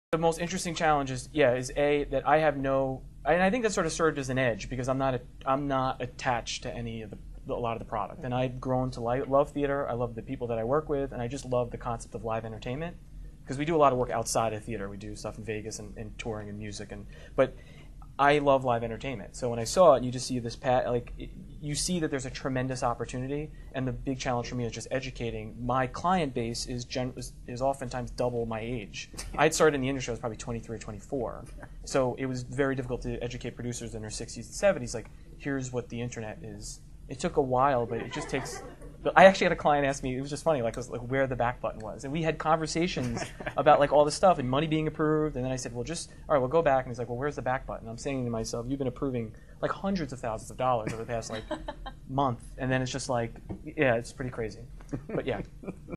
Damian Bazadona Job Tip- Center for Communication Presents Theater Publicity: Guerilla Tactics
President, Situation Marketing

Although the theater world has been slow to adopt hi-tech marketing methods, changes are taking place as a younger, tech savvy group of producers, marketers and publicists are introducing new strategies to get people into the seats. New players are dramatically changing the way shows are publicized and marketed, both on and off Broadway. Learn how these innovators are bringing in audiences.